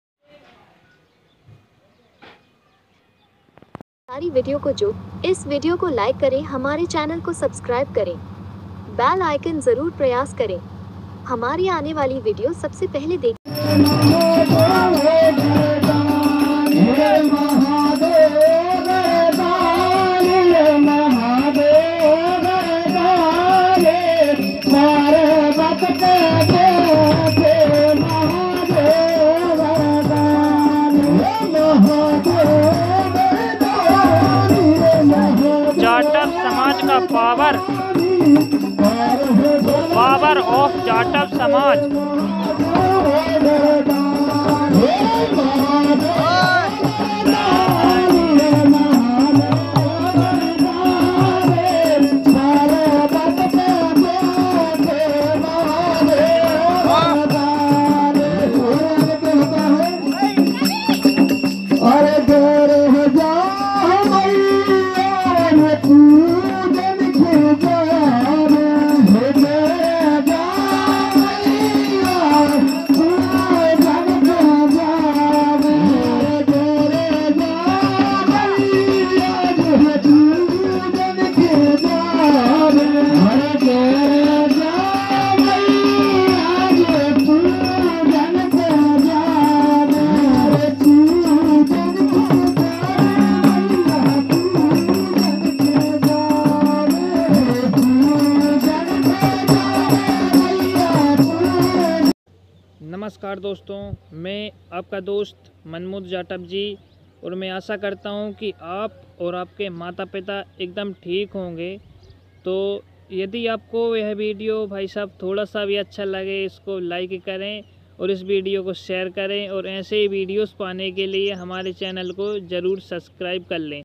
🌹🙏🌹🙏जाटव समाज का पावर तो देखो पावर ऑफ जाटव समाज 🌹🙏🌹🙏
🌹🙏🌹🙏जाटव समाज का पावर तो देखो पावर ऑफ जाटव समाज 🌹🙏🌹🙏 जाटव समाज ने ग्राम नयागांव में एकजुट होकर विधायक जी श्री मुलायम भइया को बुलाया यहां पर नयागांव में भगवान भोलेनाथ जी भगवान बजरंगबली भगवान शनि महाराज जी की स्थापना वरीया मोहल्ला में है जय जाटव समाज जय बाबासाहेब जय रविदास जी महाराज 2023s desi cow parrot animals goat dasi desii dog dogbreed जय श्री राम जय हनुमान आप सभी को राम राम